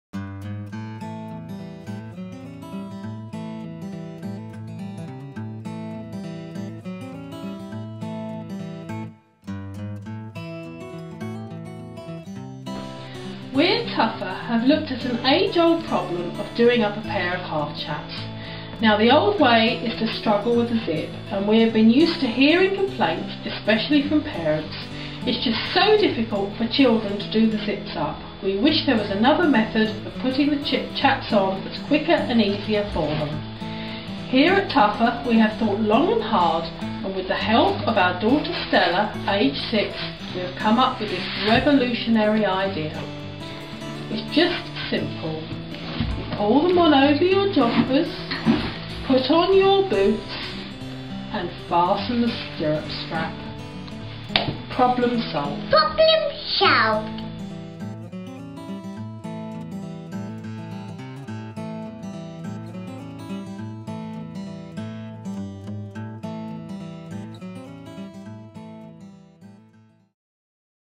Tuffa Boots - Easy Peasy Pull On Chaps
See how easy it is for children to put on by themselves, Tuffa's Easy Peasy Pull-On Chaps!
These chaps are an entirely new innovation by Tuffa.

Designed with really young riders in mind, and tested by mini riders, these lycra chaps can be pulled on easily without having to struggle with stiff zips and poppers.

All the child has to do is: Pull the chaps over boots and jodhpurs and fasten the elastic stirrup under the foot.

As you can see in our pictures, the chaps look no different to any other chap. now also available in lilac!